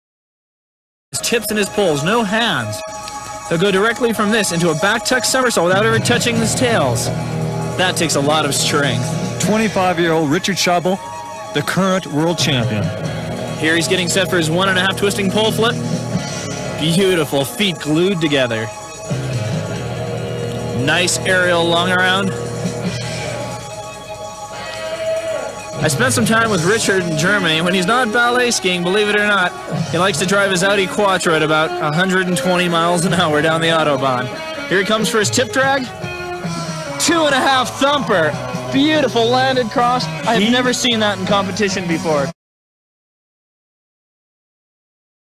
Richard Schabl Breckenridge 1985
ESPN commentaries Steven Rechtschaffner Freestyle Ski World Cup 1985 Breckenridge Colorado